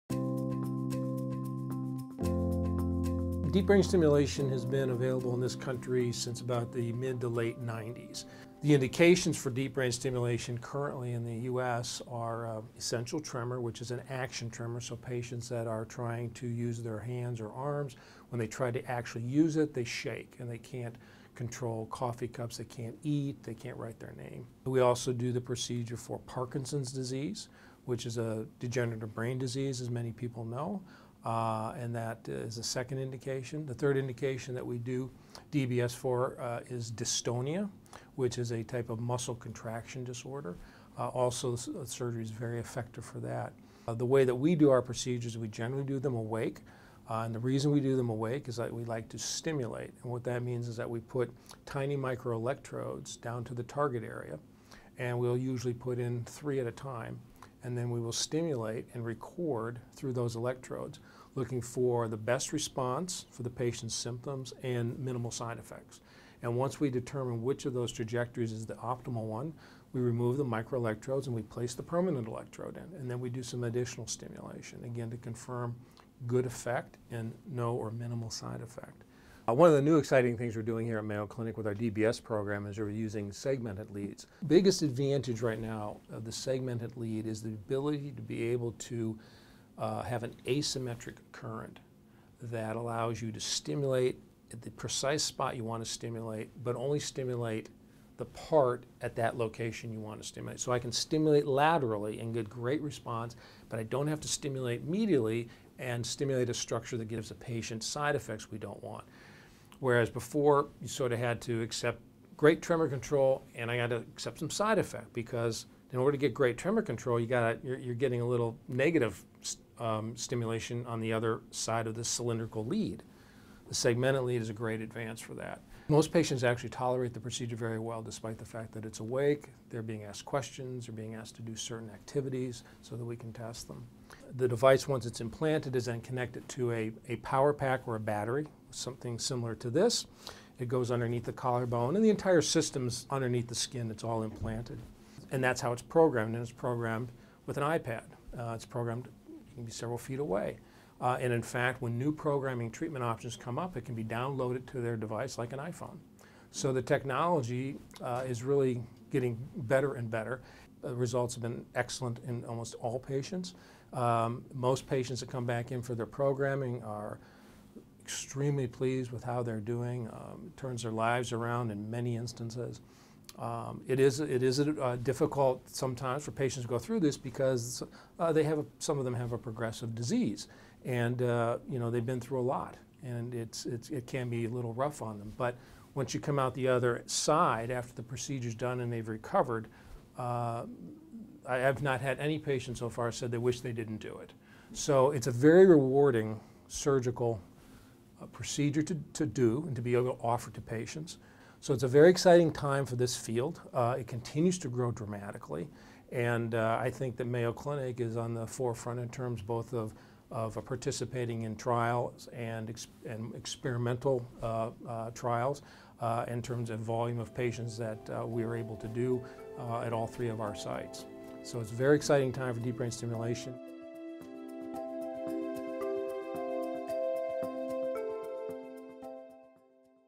Dr. Mark Lyons - Deep Brain Stimulation: Precision Using Segmented Leads
Dr. Mark Lyons, a neurosurgeon at Mayo Clinic, describes the use of deep brain stimulation as well as latest advancements including the use of segmented leads for more precision. for more information on deep brain stimulation at Mayo Clinic or to request an appointment.

Deep brain stimulation is used to treat a variety of conditions, including essential tremor, Parkinson’s disease, and dystonia.

Awake surgery for deep brain stimulation involves a surgeon putting electrodes to a target area of the brain in order to stimulate and record brain activity. This helps the surgeon achieve the best response to manage patients’ symptoms, with minimal side effects.

Mayo Clinic is pioneering segmented leads for deep brain stimulation, which allows for an asymmetric current. This allows for more precise stimulation of a specific area of the brain, helping reduce side effects.